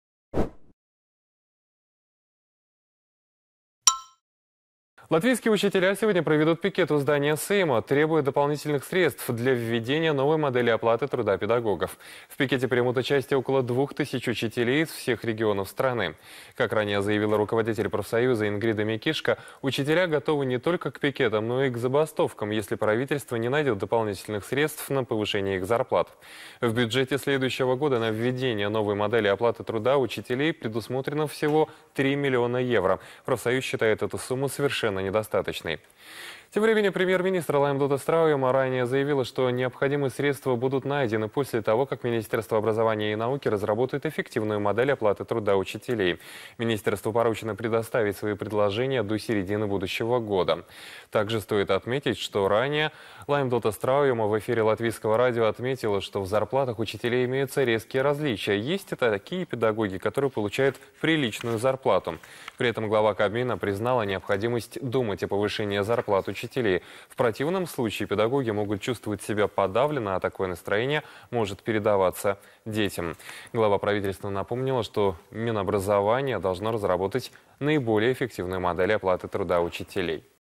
Повторение - мать учения
Латвийские учителя сегодня проведут пикет у здания Сейма, требуя дополнительных средств для введения новой модели оплаты труда педагогов. В пикете примут участие около 2 тыс. учителей из всех регионов страны.